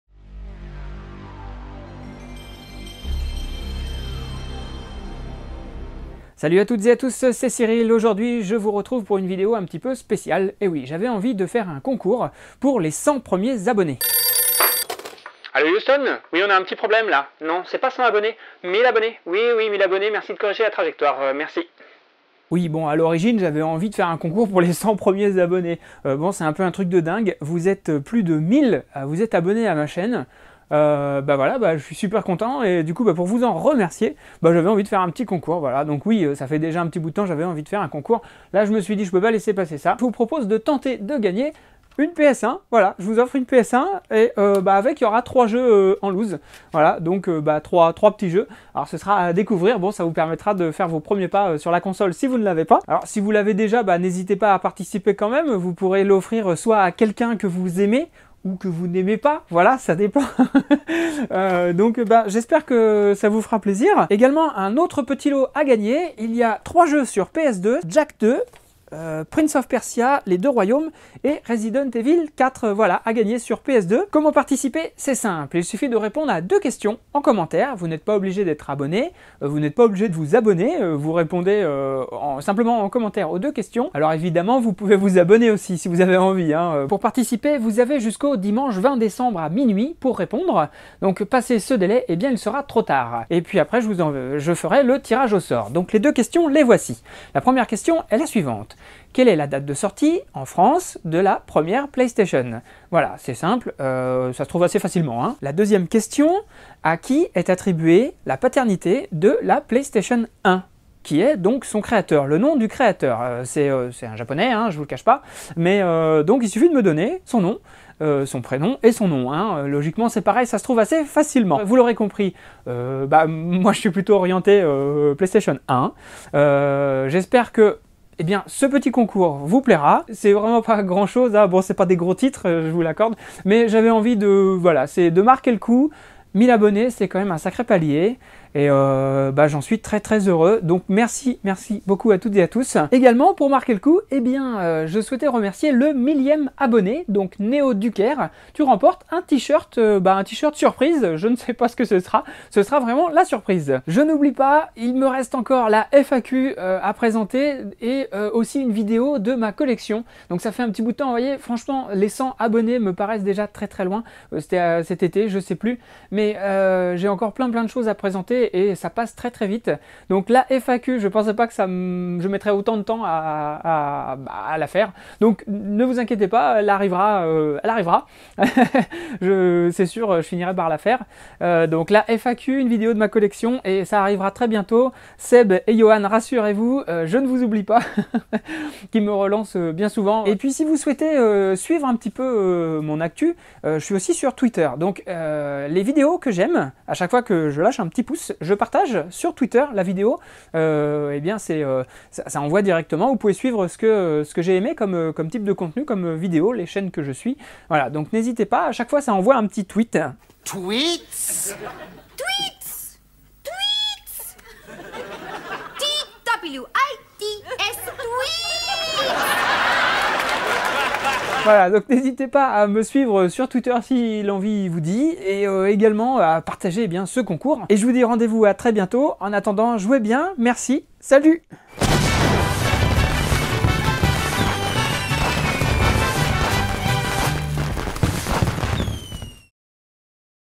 retro gaming PS1 - CONCOURS #1 - 1000 ABONNES (terminé)
merci aux 1000 abonnés de ma chaine ! pour participer au concours : il suffit de répondre en commentaire aux 2 questions posées dans la vidéo avant le Dimanche 20 Décembre 2015 minuit ! les gagnants seront tirés au sort parmi les bonnes réponses.
me contacter de préférence par mail : pocosso à gmail point com